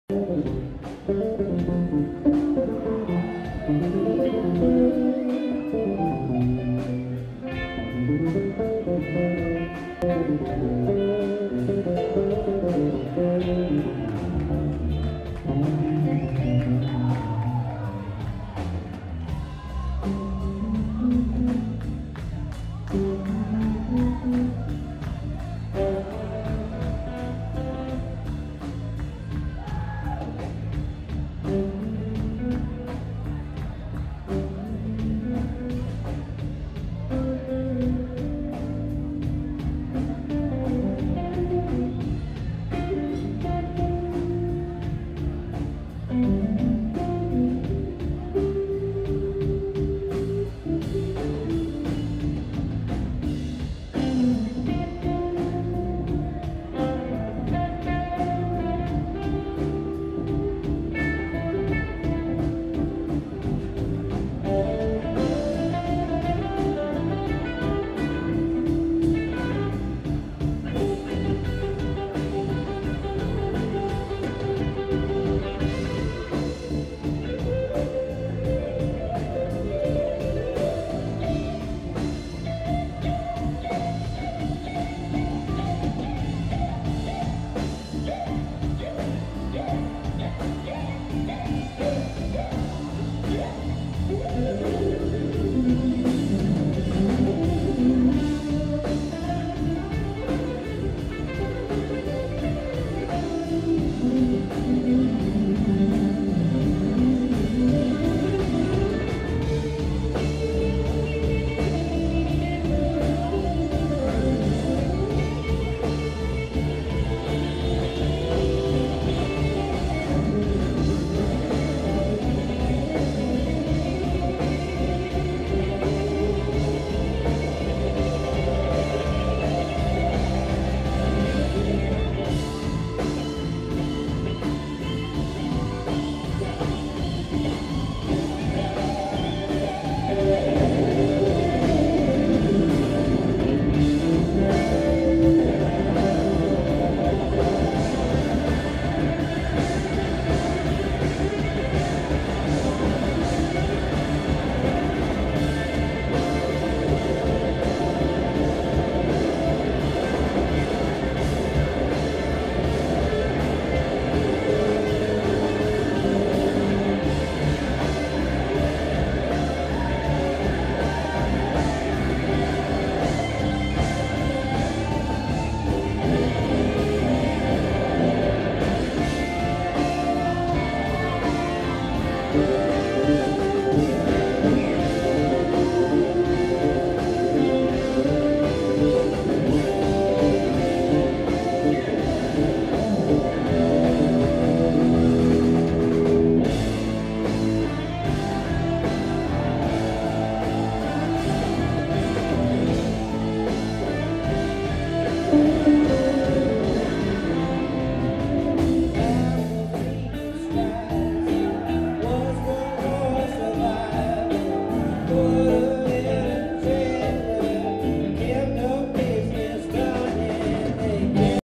TLG 3-11-10 insane green jam Reed Mathis bass into Josh Clark lead guitar
Tea Leaf Green jam from Devil's Pay March 11, 2010, Reed Mathis (bass) and Josh Clark (lead guitar), live video webcast recorded (find more vids like this on youtube @colbruce and in the ustream archives @dgold)